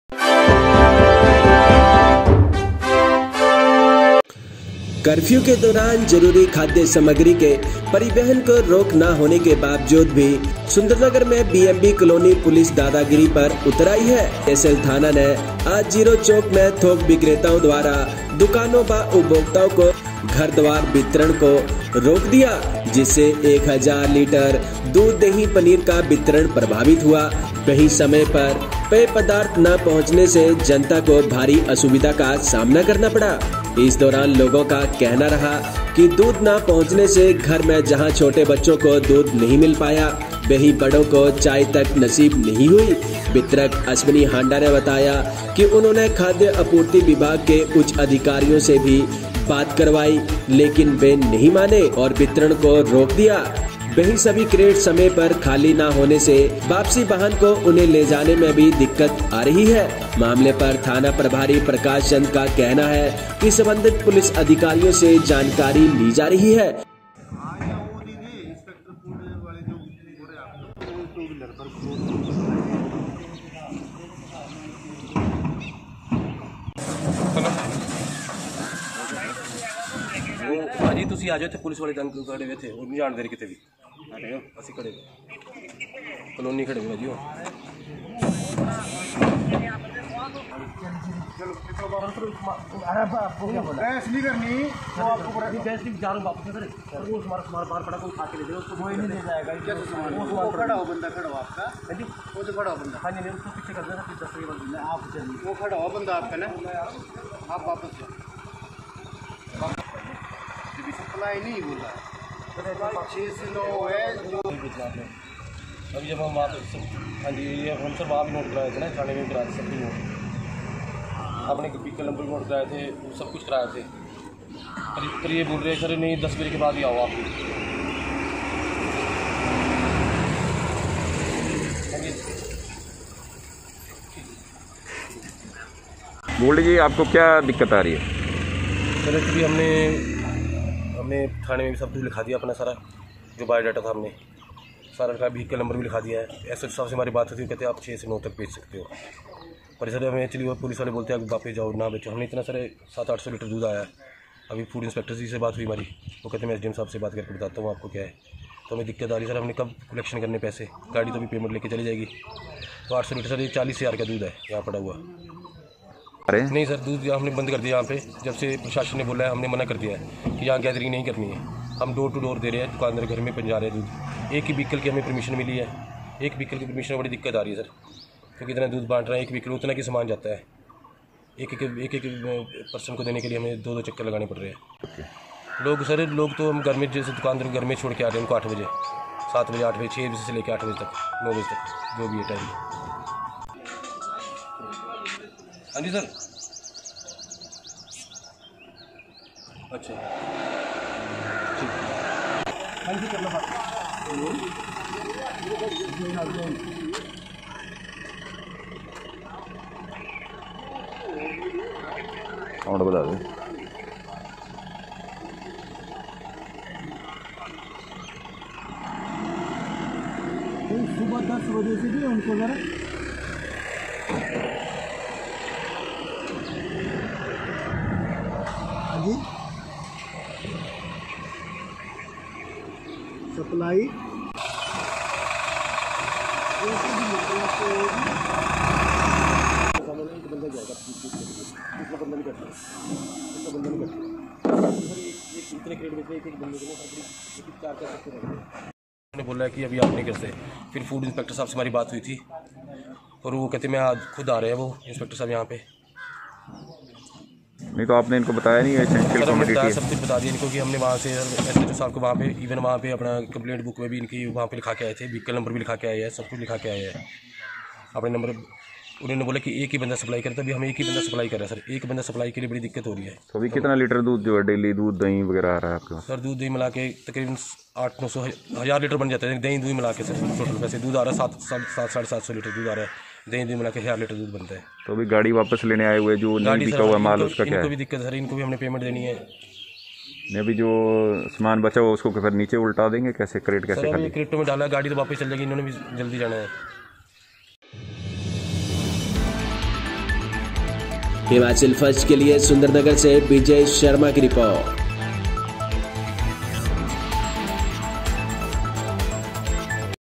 1000 लीटर दूध,दही,पनीर का घर द्वार नही हो पाया वितरण,जनता परेशान
1000 लीटर दूध,दही,पनीर का घर द्वार नही हो पाया वितरण,जनता परेशान